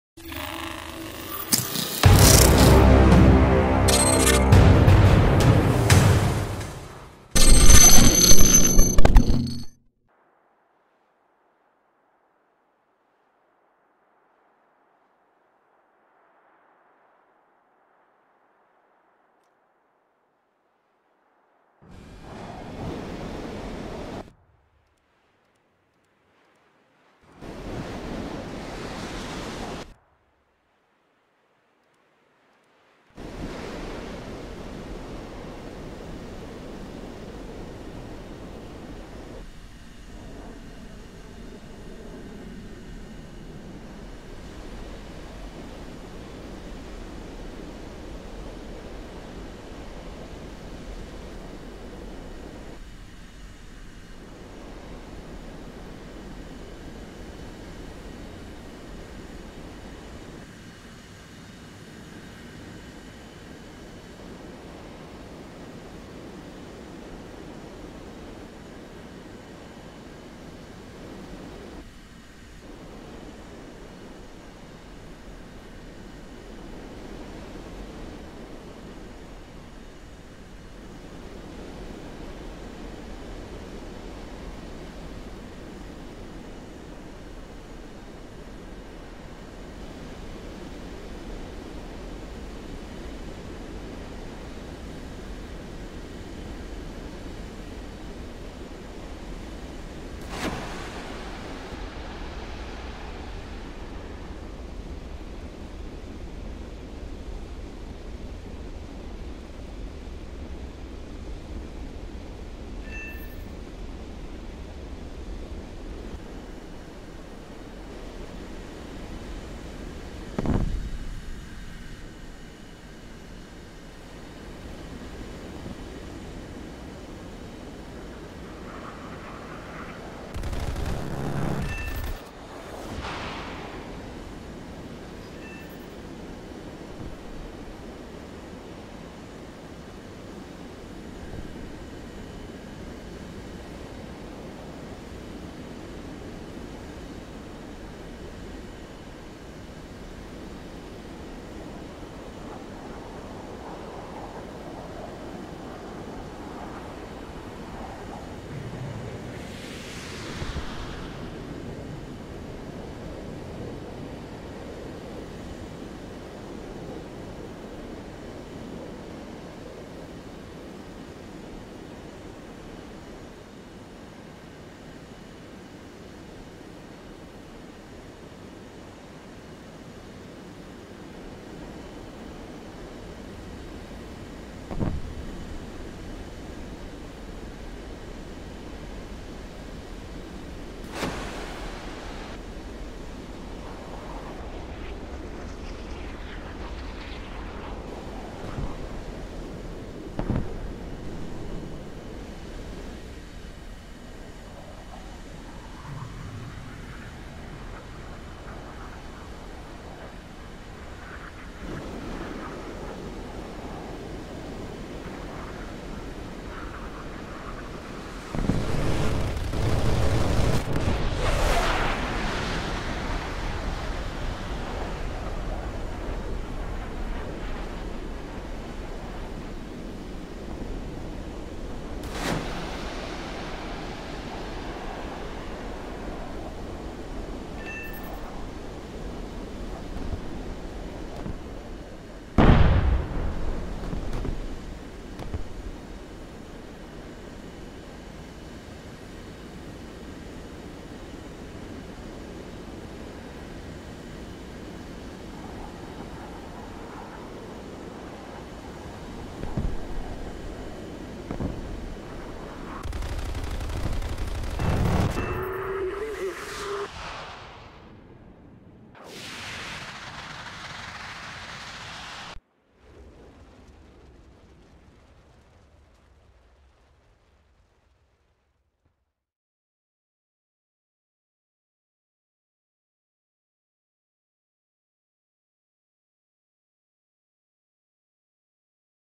Flight of the Crusader
Just got my Aim-9Ds and was very satisfied with this match, enjoy!!